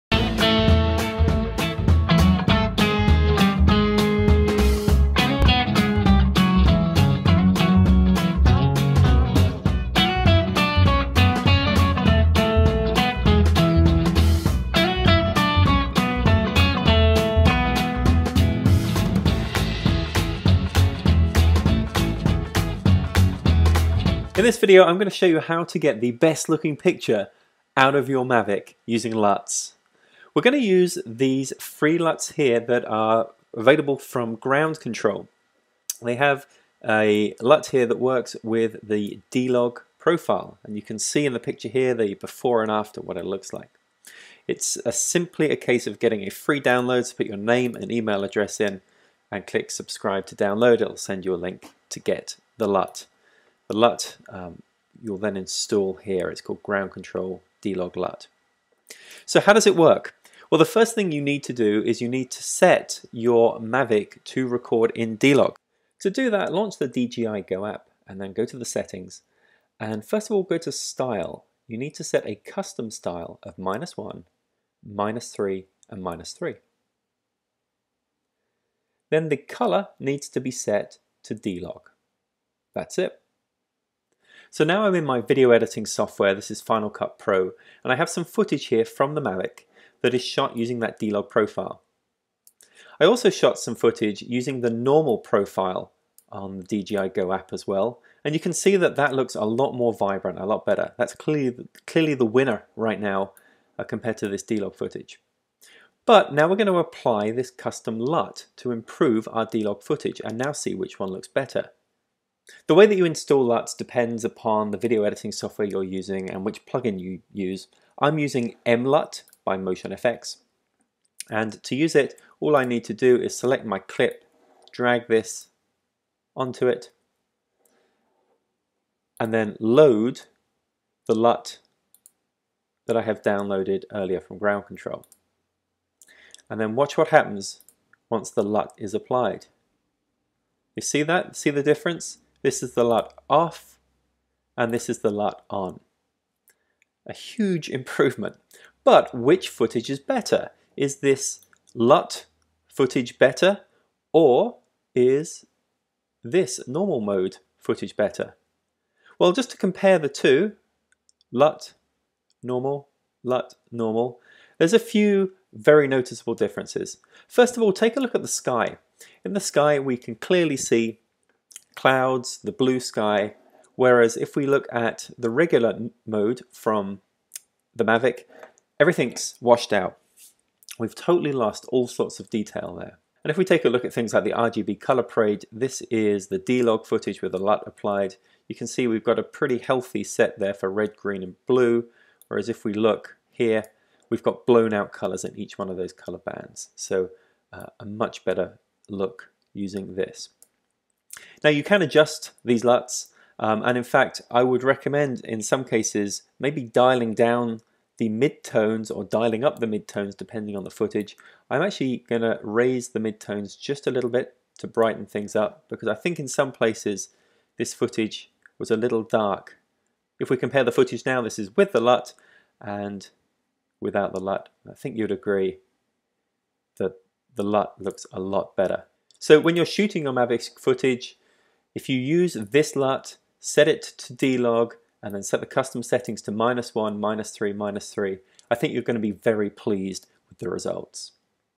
DJI Mavic Pro: Best video settings using LUTs and D-Log
The DJI Mavic Pro offers many video settings. In this video we compare a few options, and show the power of utilizing LUTs.

Link for the free Ground Control LUT for DJI Mavic Pro